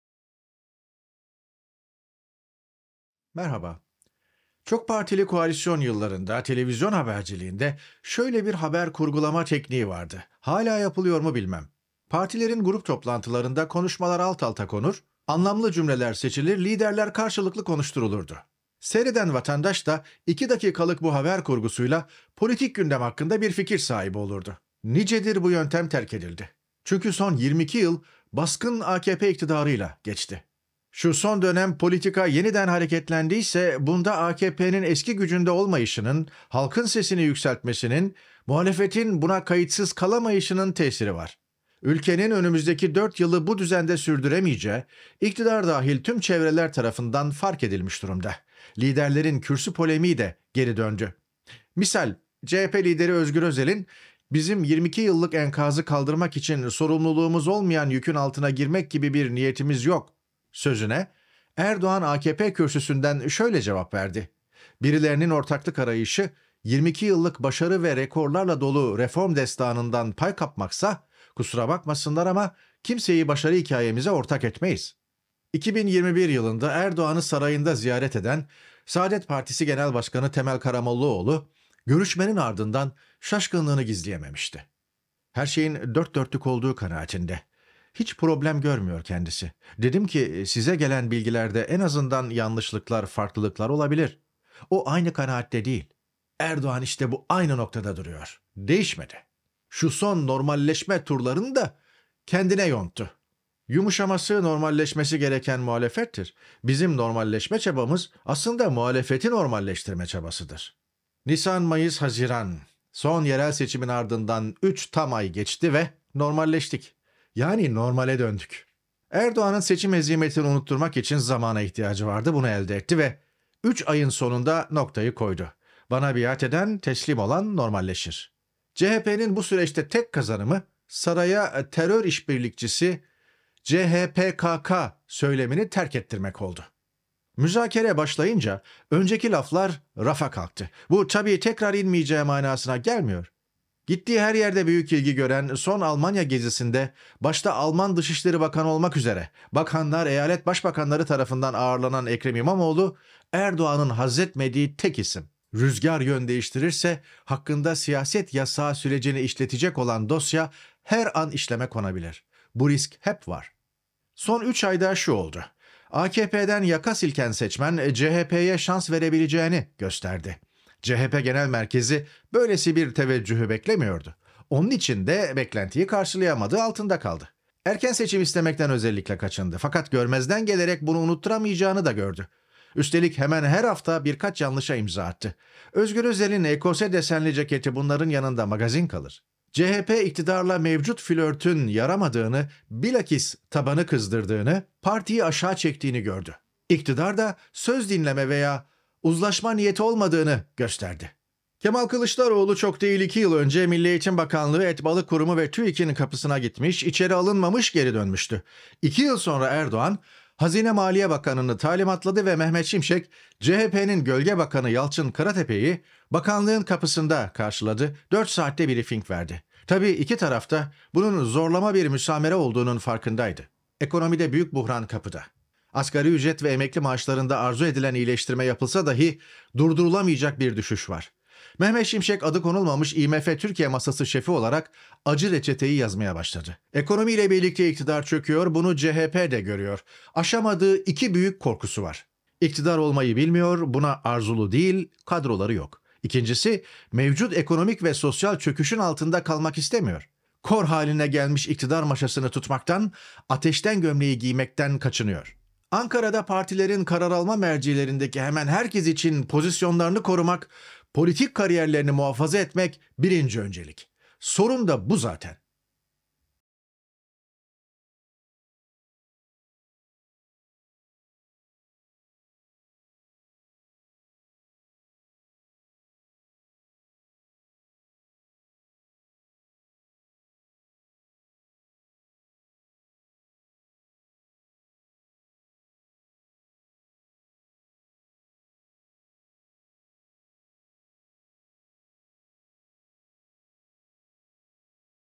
Dip dalga: İktidar çökerken iki mühim korku.. | Tarık Toros | Analiz | 30 Haziran 2024
30 Haziran 2024

• Erdoğan’ın seçim hezimetini unutturmak için zamana ihtiyacı vardı, bunu elde etti ve 3 ayın sonunda noktayı koydu: Bana biat eden, teslim olan normalleşir!
• CHP’nin bu süreçte tek kazanımı, Saray’a “terör işbirlikçisi-CHPKK” söylemini terk ettirmek oldu.
• CHP, iktidarla mevcut flörtün yaramadığını bilakis tabanı kızdırdığını, partiyi aşağı çektiğini gördü. İktidar da, söz dinleme veya uzlaşma niyeti olmadığını gösterdi.
• Ekonomide büyük buhran kapıda. Asgari ücret ve emekli maaşlarında arzu edilen iyileştirme yapılsa dahi durdurulamayacak bir düşüş var.
• Mehmet Şimşek, adı konulmamış IMF Türkiye Masası Şefi olarak acı reçeteyi yazmaya başladı.
• Ekonomiyle birlikte iktidar da çöküyor. Bunu CHP de görüyor. Aşamadığı iki büyük korkusu var
• Ankara’da partilerin karar alma mercilerindeki hemen herkes için, pozisyonlarını korumak, politik kariyerlerini muhafaza etmek birinci öncelik.